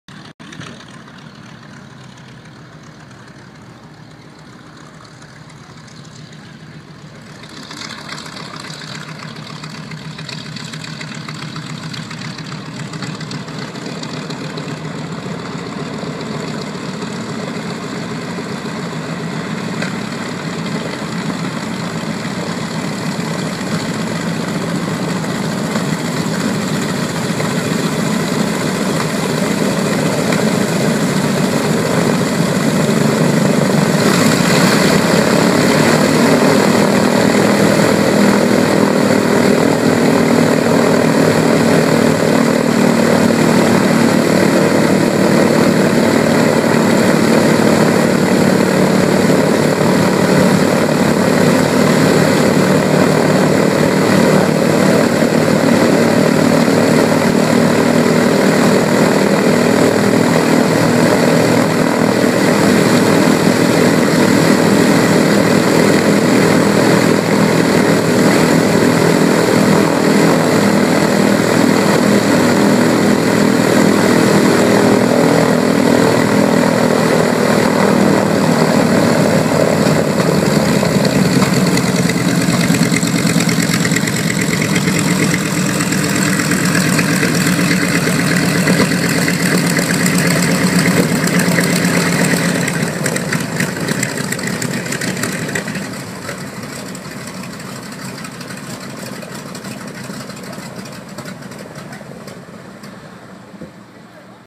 B-25 Maid in the Shade into Victoria Flying Club, June 2017
Flight of Maid in the Shade returning to Victoria Flying Club after a 20 minute tour of Southern Vancouver Island with veterans Reg Price, Garry Brown and Connie Barr as passengers.